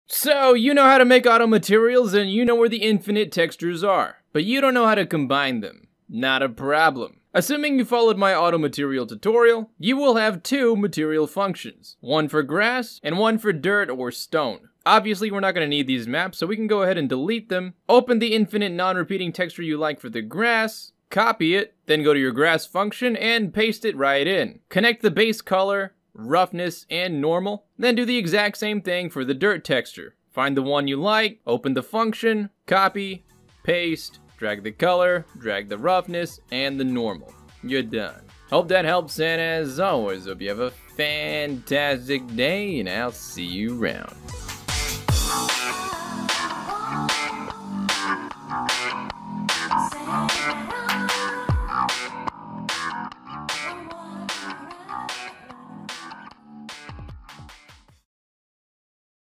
Unreal 5 - Non-Repeating AUTO-Materials (60 SECONDS!!)
Here's the easiest way to get infinite "Non-Repeating" texture maps you can use for Auto-Materials while sculpting your environments in Unreal-5!!